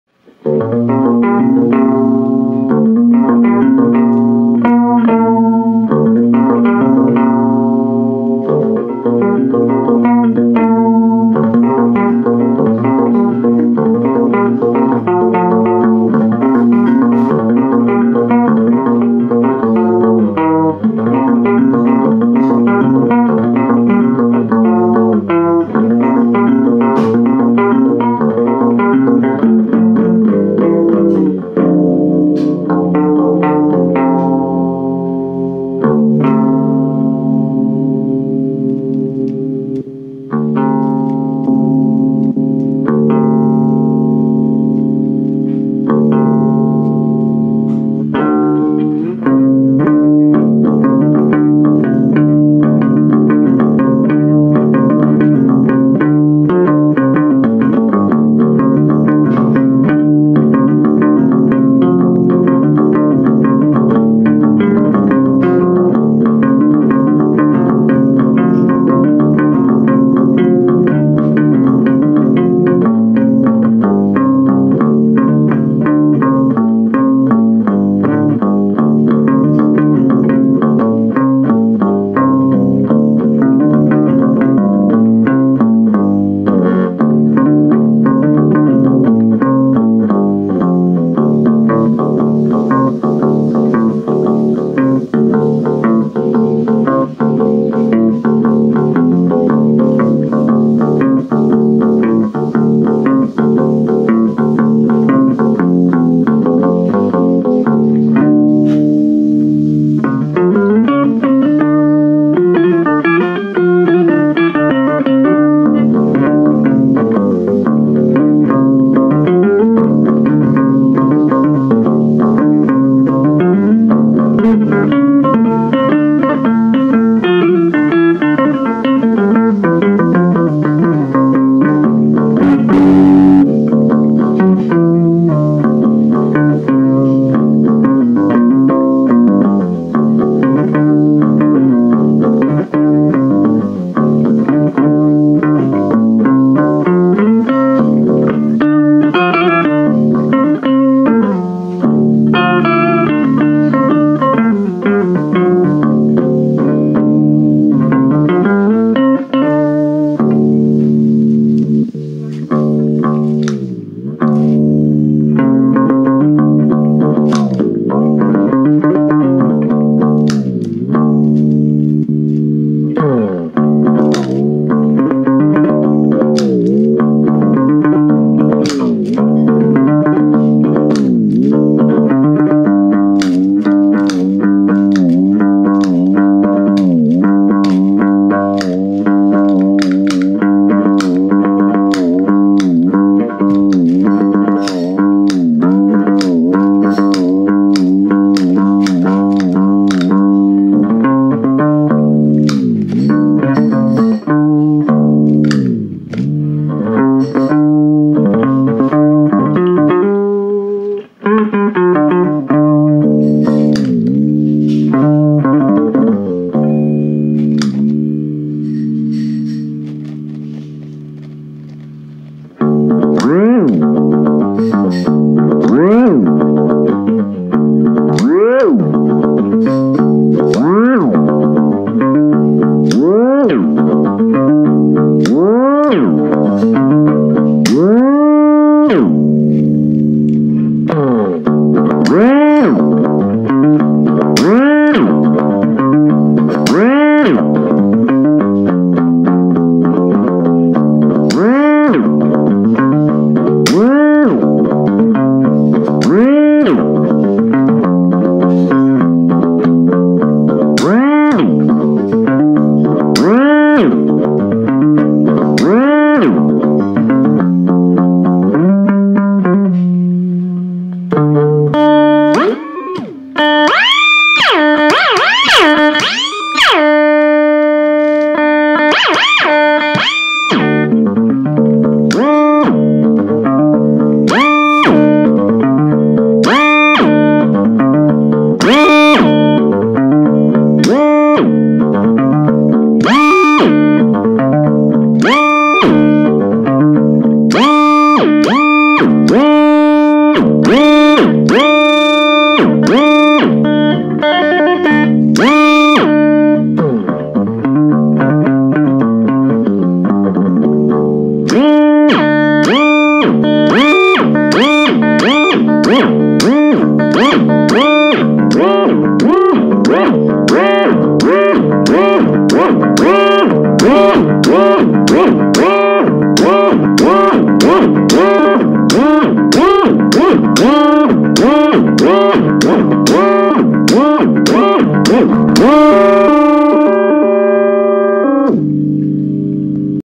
IS THE BOSS PS-5 SUPER SHIFTER A MULTI FX PEDAL???
super shifter can do octave, harmonizer, chorus, delay and whammyish stuff. is it an undercover multi effect??? kinda